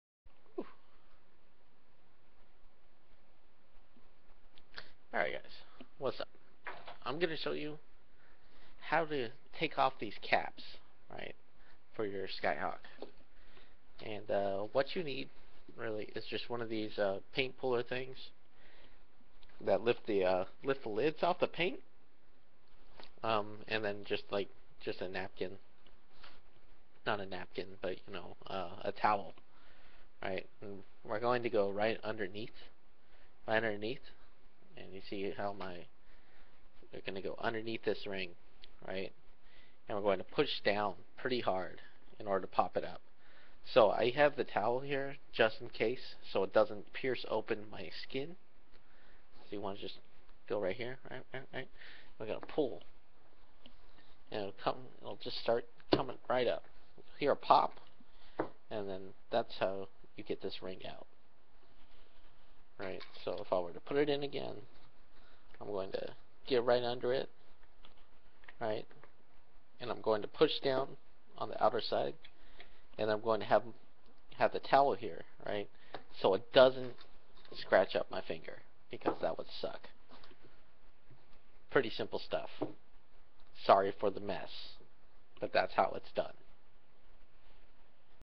Yoyo Maintenance (Removing Weight rings)
Player: Sam Lopez
Yoyo Used: Duncan Skyhawk
Difficulty: 5/10

This video is how to remove the weight rings from the duncan skyhawk without cutting open your hand!

Use the towel to cover the hand you are holding the yoyo with!
Use a tool to get under the weight ring and press down firmly.

Questions? comments? post away!